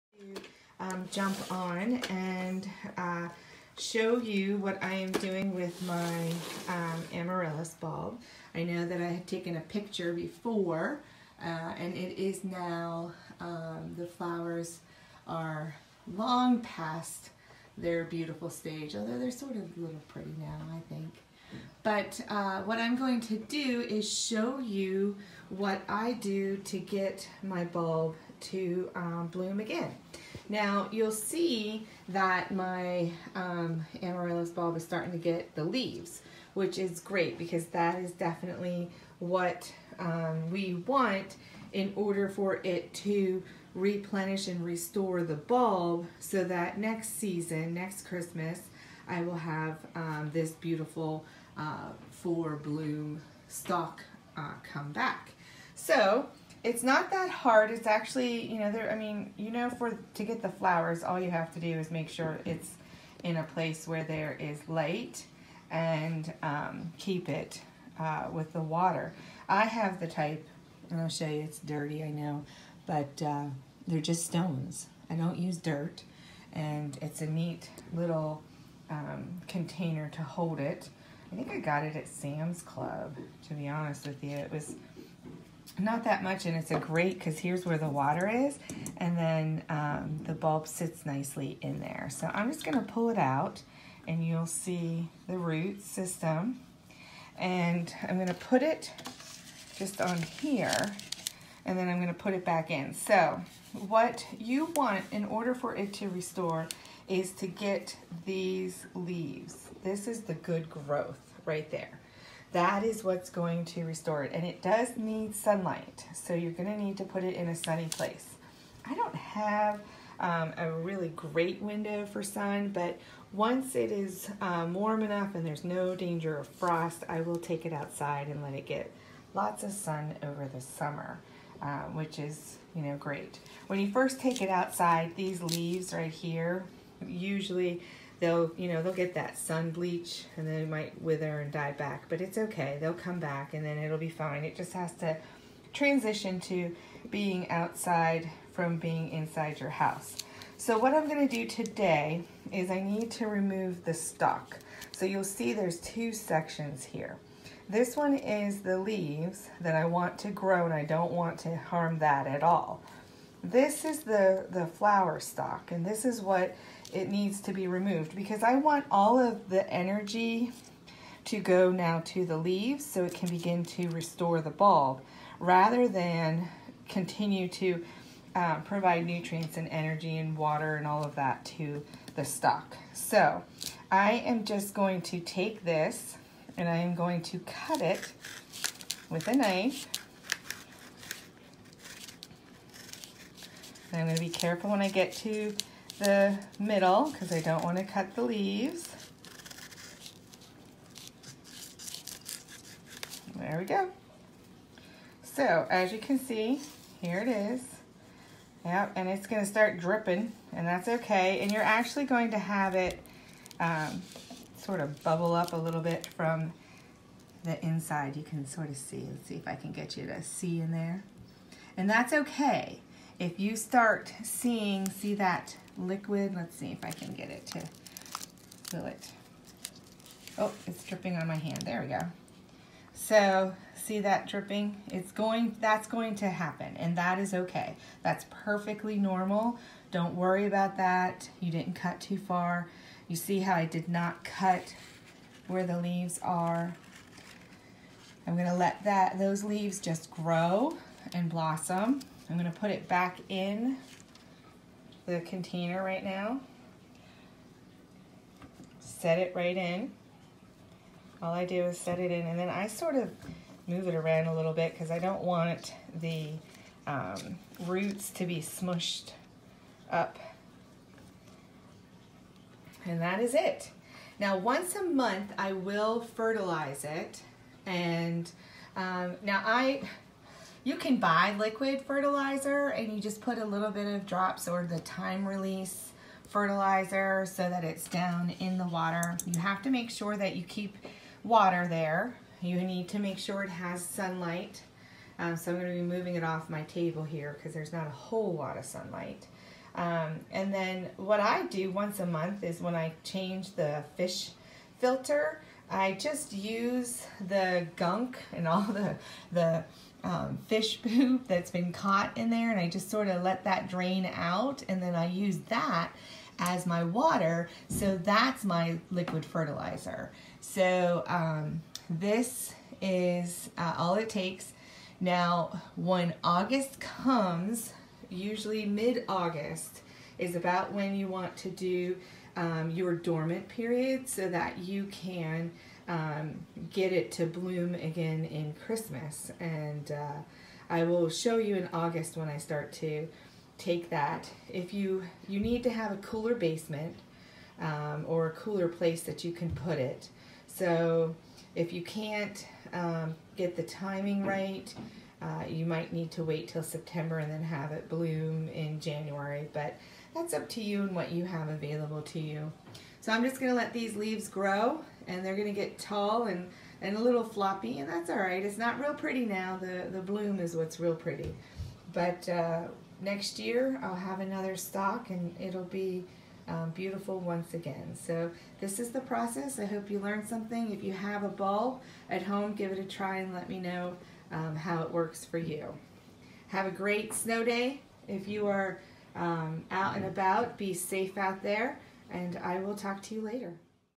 How to Get your Amaryllis to Bloom Again
Many of us have received Amaryllis plants for the Holidays- Now what do you do once the blooms die? Amaryllis is largely regarded as a throw away plant, but with a little care this lovely plant can bloom for you for years to come.  This video will take you through the process of after flowering care, fertilizing, culture and care to re-flower these majestic beauties in the home year after year.